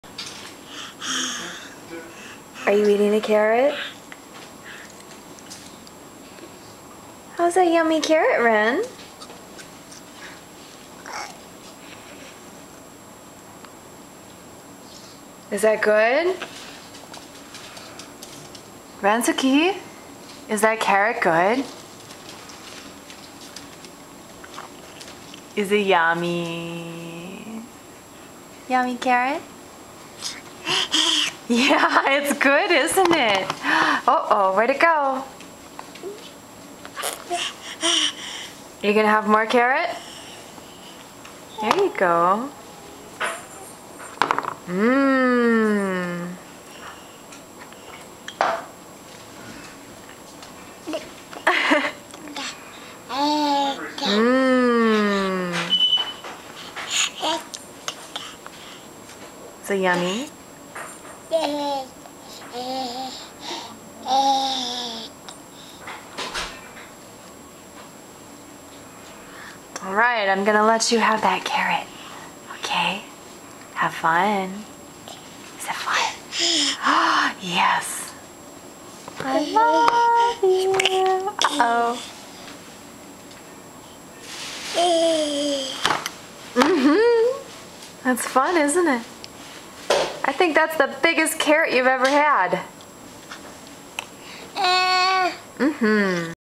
what's up doc
wren eating a carrot